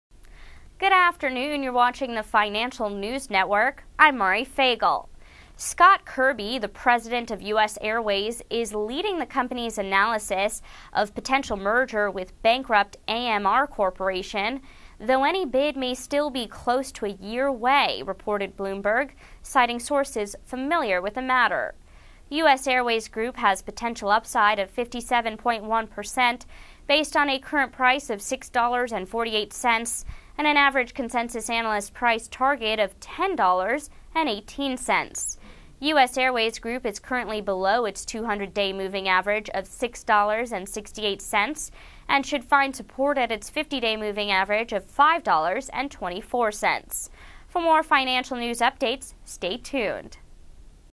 President of US Airways Leading Study of AMR Bid (LCC)
Scott Kirby, the President of US Airways (NYSE:LCC) is leading the company's analysis of potential merger with bankrupt AMR Corp., though any bid may still be close to a year away, reported Bloomberg, citing sources familiar with the matter.

US Airways Group (NYSE:LCC) has potential upside of 57.1% based on a current price of $6.48 and an average consensus analyst price target of $10.18.

US Airways Group is currently below its 200-day moving average (MA) of $6.68 and should find support at its 50-day MA of $5.24.